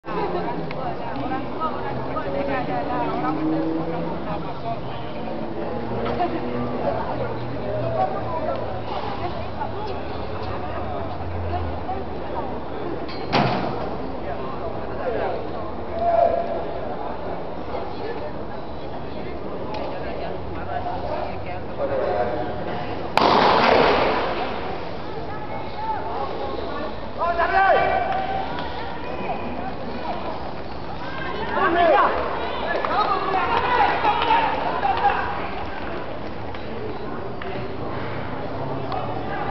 25th Malaysian Open Masters Athletic Meet 2011 - 100m (Men-40yrs)
Venue: Darul Aman Stadium, Alor Setar Kedah. 1-2 October 2011